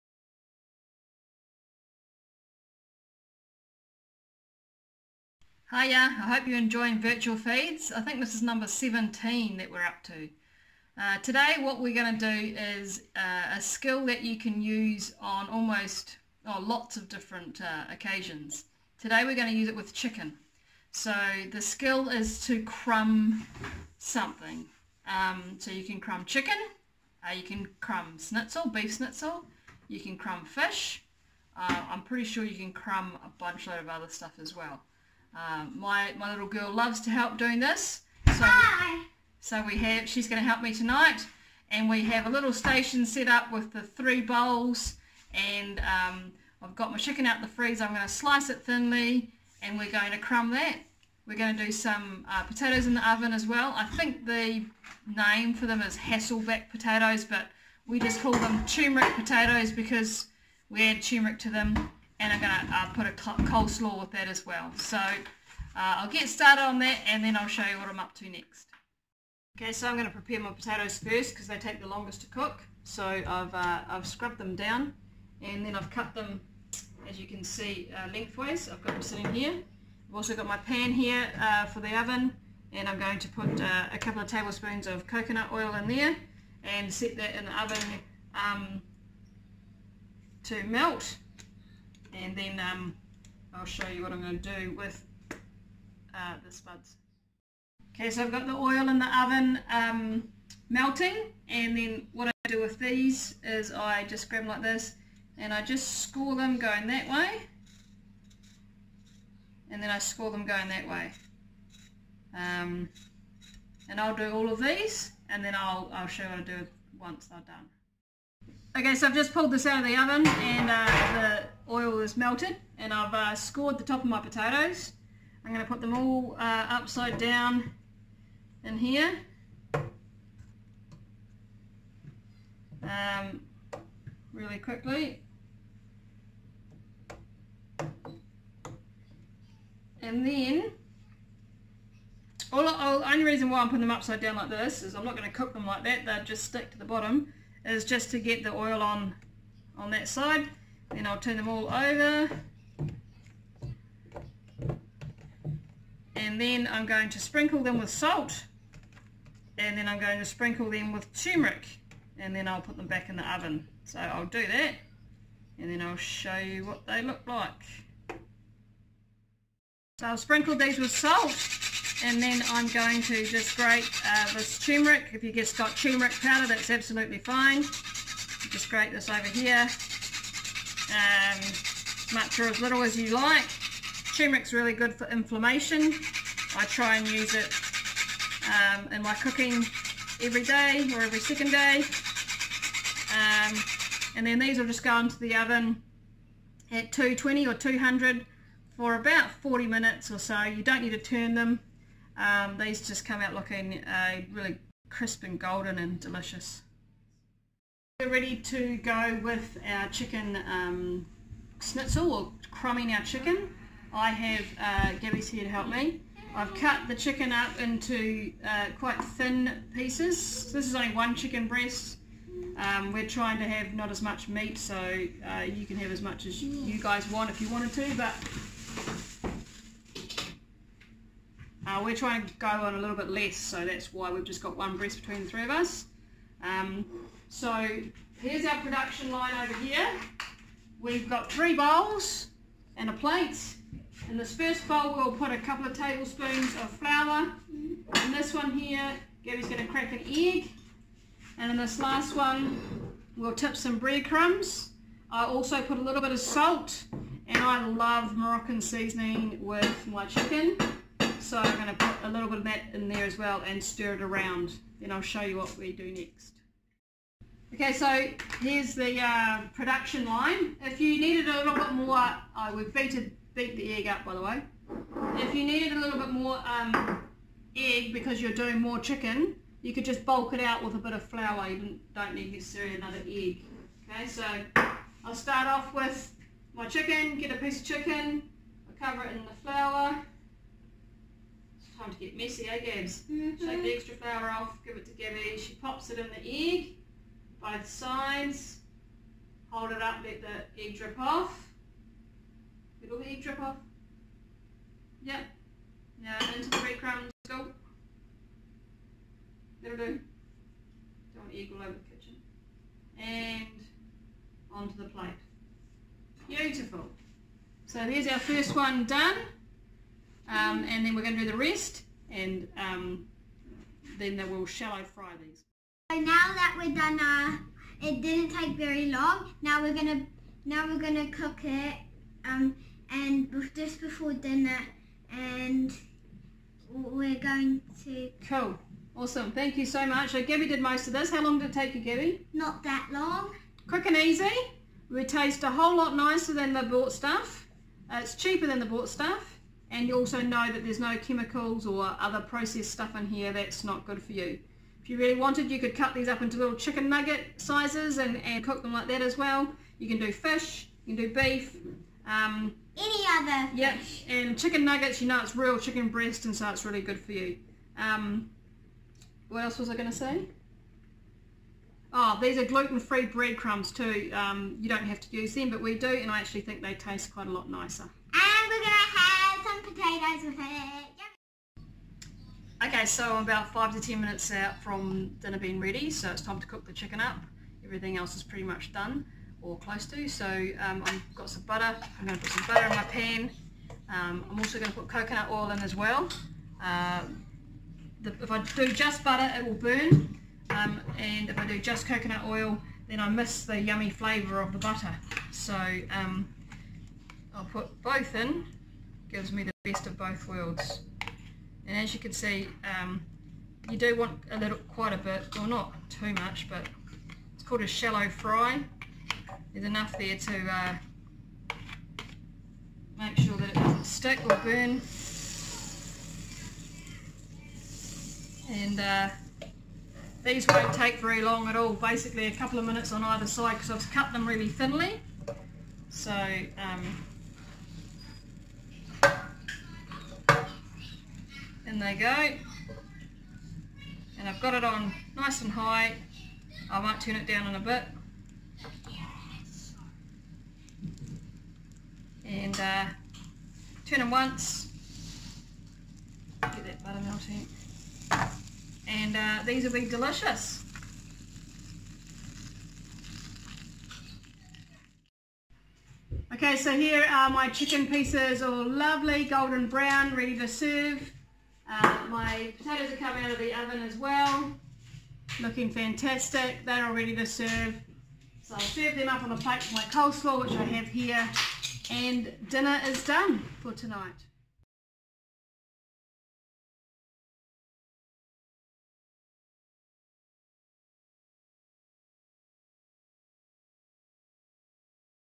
Virtual Feeds #17 - Crumbed Chicken & Tumeric Potatoes
A simple but yummy crumbing technique used with chicken (they could also be for chicken burgers) served with Tumeric Potatoes and coleslaw.